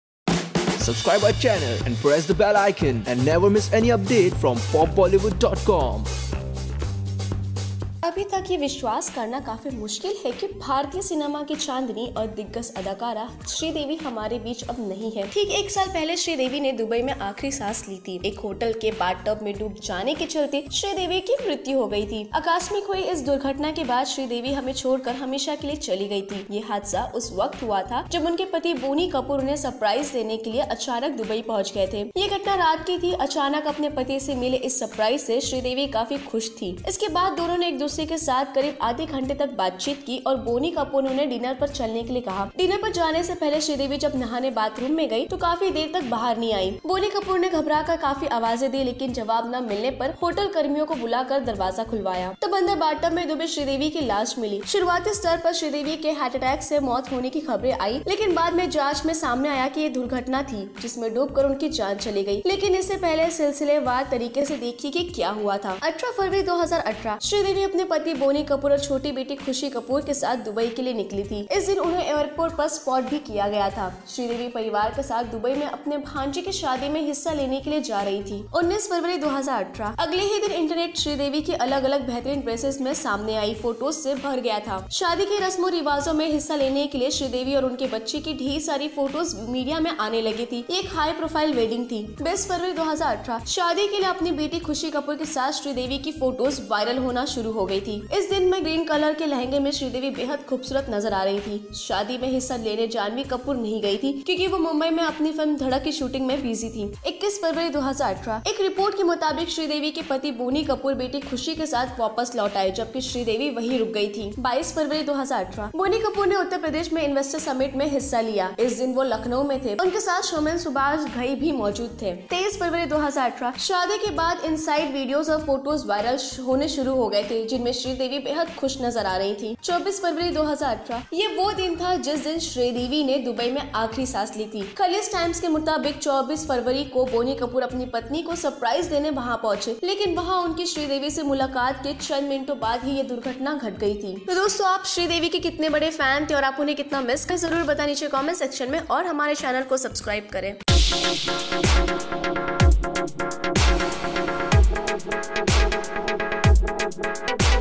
Sridevi जाने आखिर कैसे हुई थी Sridevi की मौत
Sridevi Death Anniversary - Sridevi, Bollywood's first female superstar, departed for her heavenly abode a year back on February 24, 2018. Her death, by way of accidental drowning in a bathtub at a Dubai hotel, left the film industry, her family and legions of fans in a state of shock.